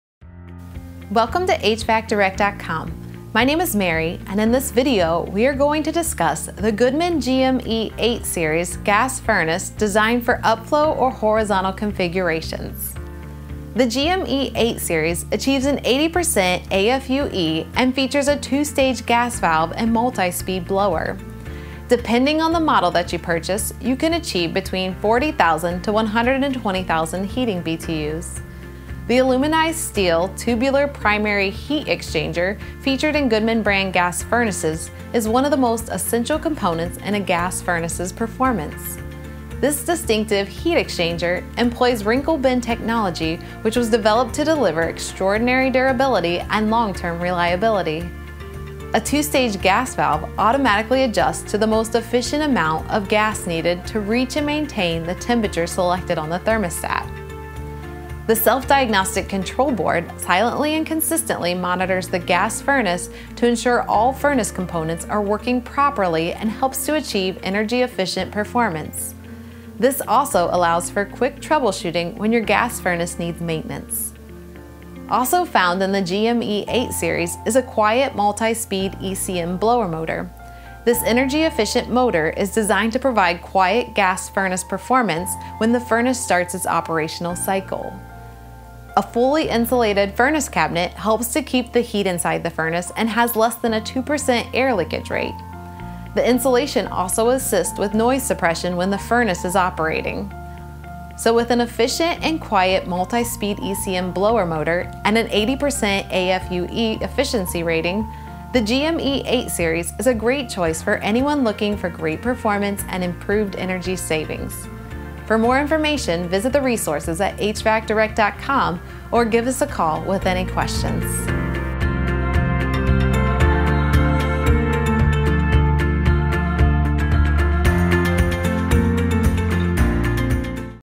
Goodman GME8 Series 80% AFUE Furnace
The Goodman GME8 series furnace features a two stage gas valve and multi-speed blower motor that can be installed in either upflow or horizontal configurations. This furnace also features a X13 blower motor that saves electricity during the heating and cooling months. This direct current motor uses less electricity than the standard multi-speed furnace blower. This furnace also includes a patented aluminized-steel tubular heat exchanger and durable Silicon Nitride Hot Surface Ignition system.